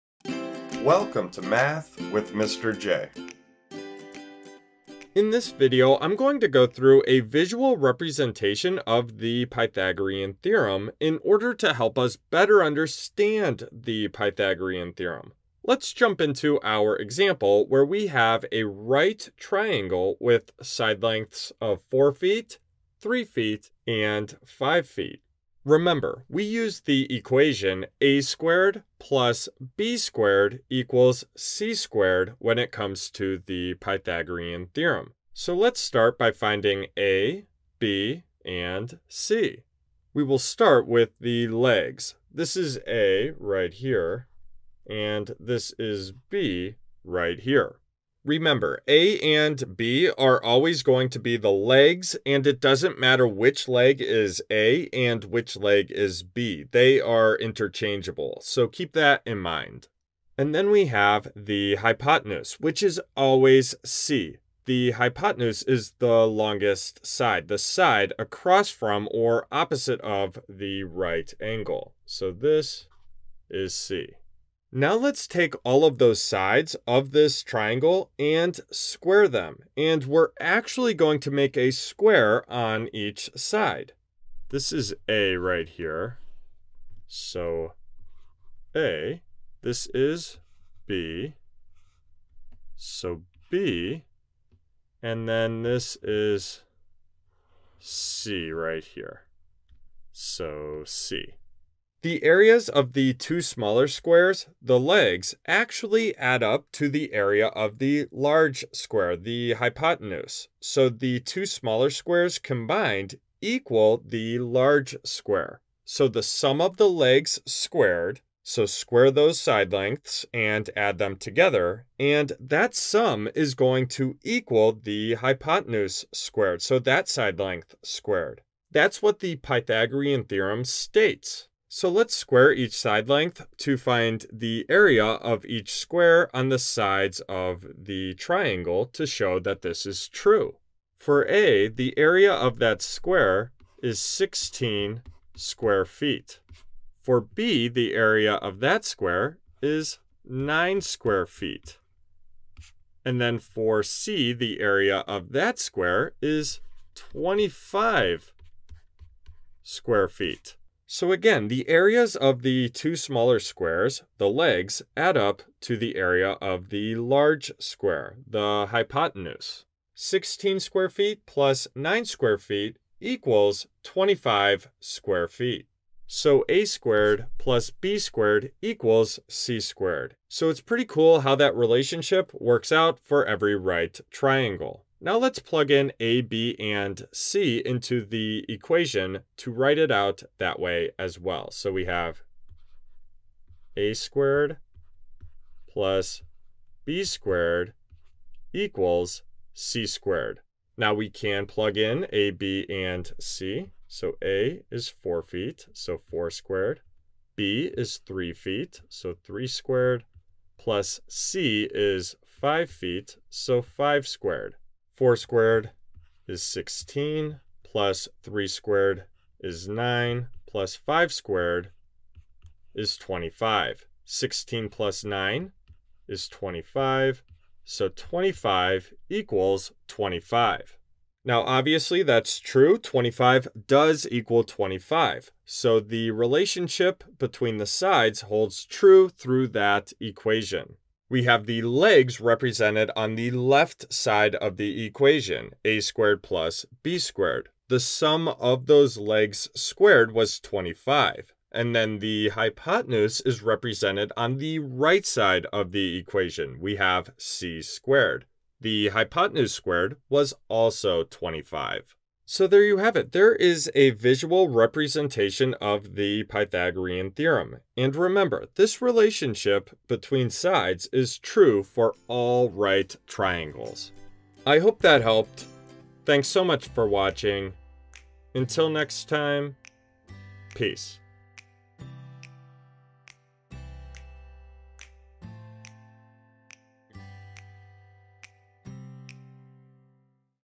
Visualizing the Pythagorean Theorem | Math with Mr. J
Welcome to Visualizing the Pythagorean Theorem with Mr. J! Need help with the Pythagorean Theorem? You're in the right place!

Whether you're just starting out, or need a quick refresher, this is the video for you if you're looking for help with the Pythagorean Theorem. Mr. J will go through an example of a right triangle and explain the Pythagorean Theorem.

Hopefully this video is what you're looking for when it comes to the Pythagorean Theorem.